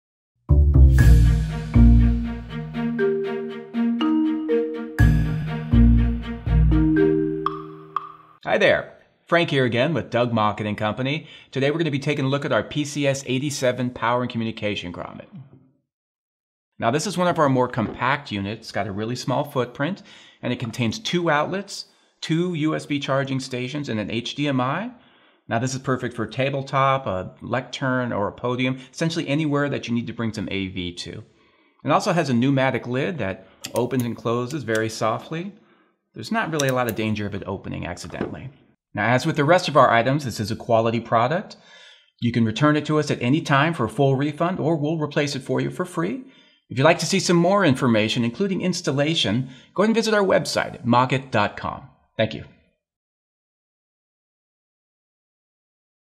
New Part! Flip-Up Power Grommet PCS87
Who knew you could pack so much connectivity into such a small footprint? Unit includes two Power outlets, HDMI, and two 2A USB chargers all neatly configured in this low profile unit. Easy plug and play installation with a standard power cord for the Power and USB and the HDMI connector cabling comes pre-wired with a female connector. Perfect for conference room tables, lecterns and podiums, or anywhere a/v hookups are needed to power a presentation for a group on the big screen. Hook up a laptop and project your image to a larger screen all while powering your laptop, phone, or other devices.

• Press to flip up, press down to close
• 2 Power, 2 USB Chargers, 1 HDMI
• Soft close spring-loaded cap
• Satin Aluminum body
• UL Listed